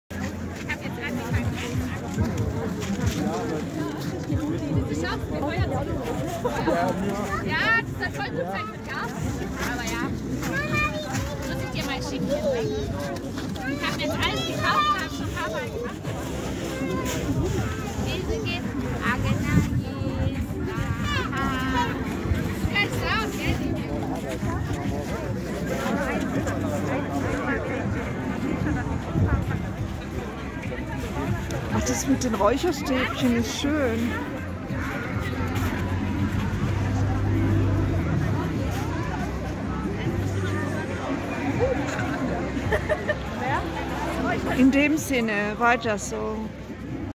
Lindau Protest gegen Corona-Massnahmen.
Lichter für die Erhellung der Politiker.
Für den Frieden und die Freiheit in unserem Land
stehen die Menschen auf.